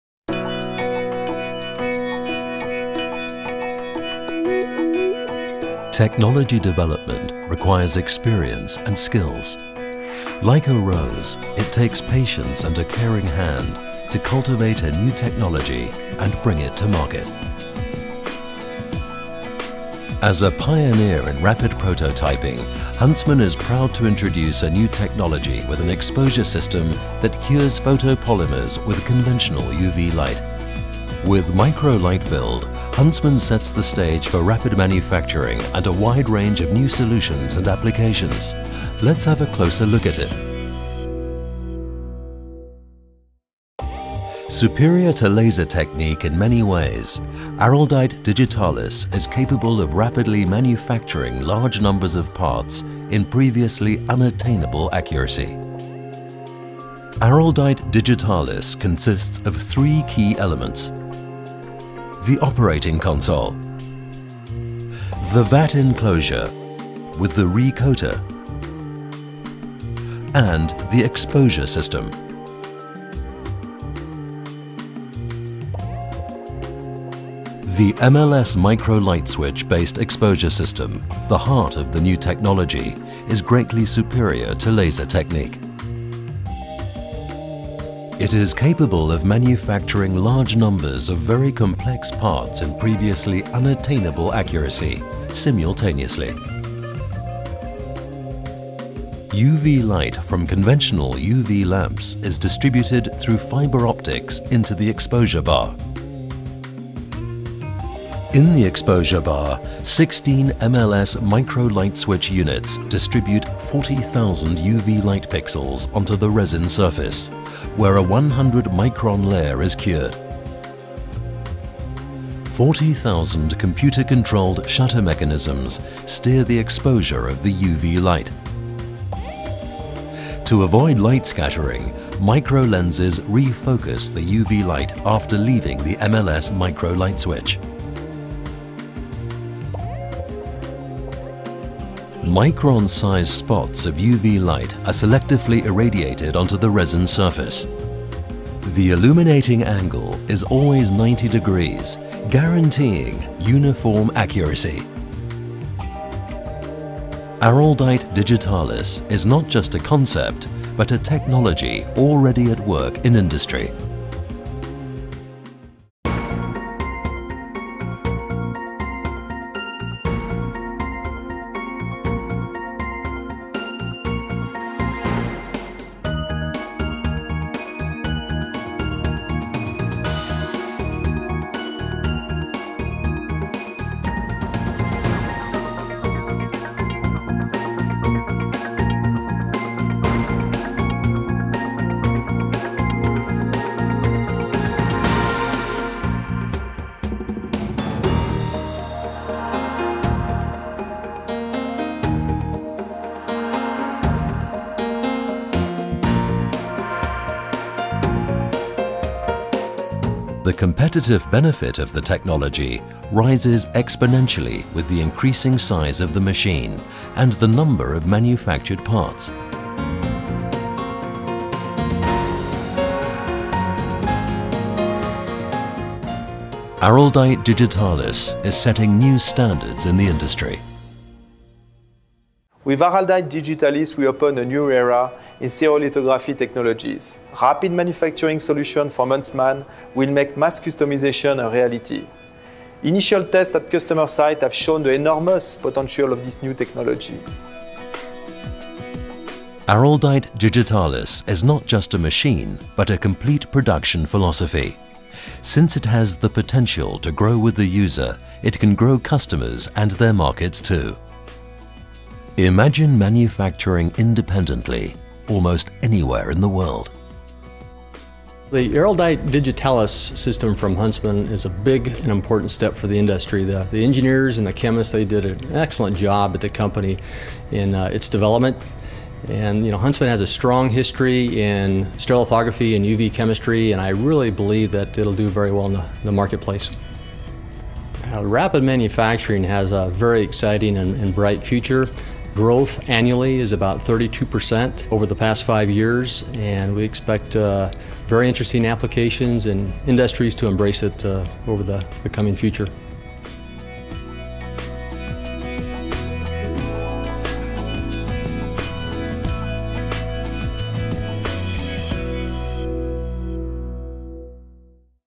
Huntsman Advanced Materials - Araldite Digitalis Rapid Manufacturing System
Video showing the new and groundbreaking Araldite Digitalis Rapid Manufacturing System from Huntsman Advanced Materials